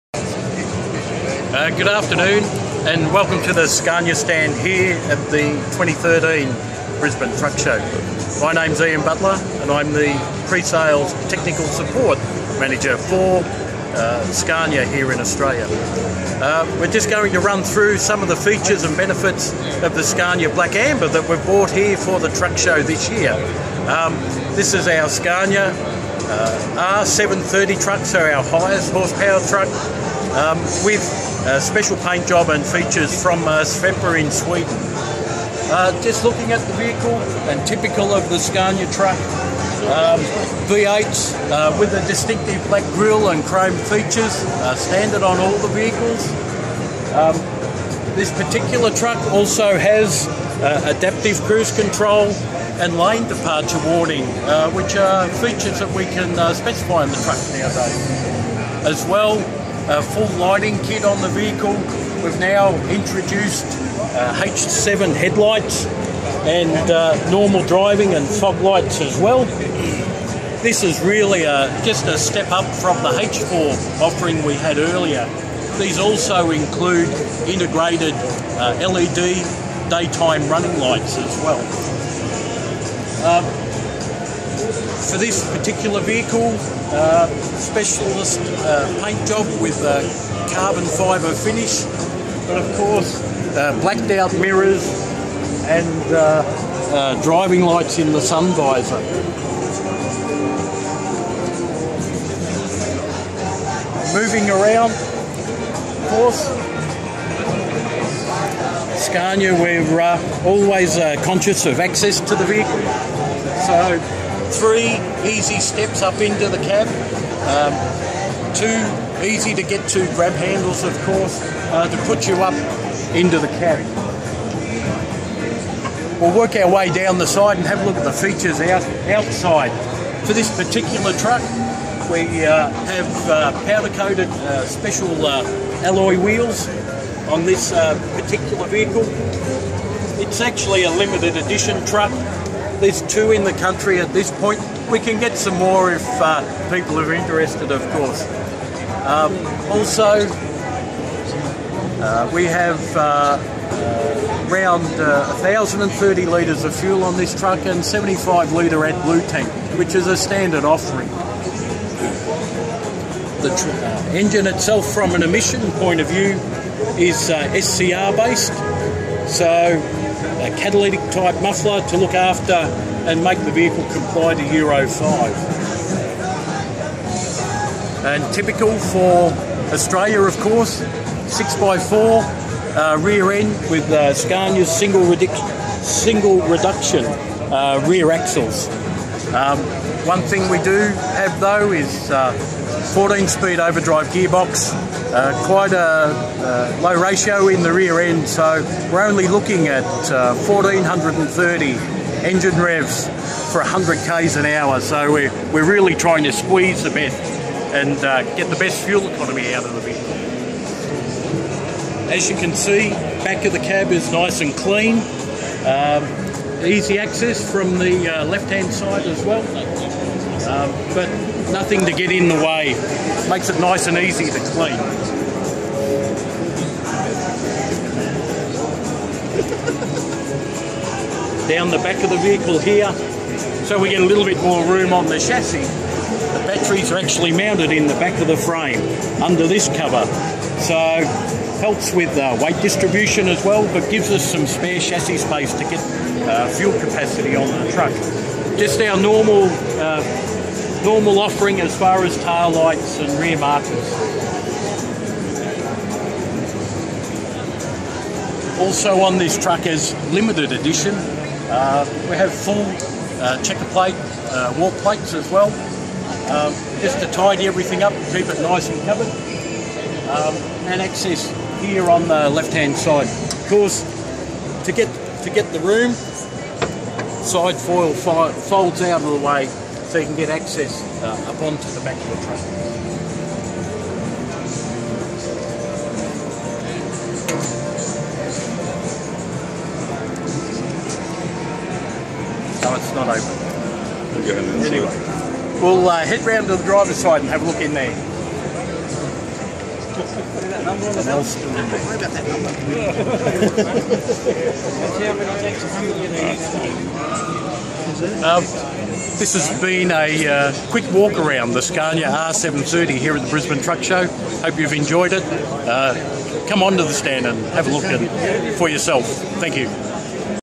Scania R730 Prime Mover walk around
Quick walk around the new Scania R730. Brisbane truck show 2013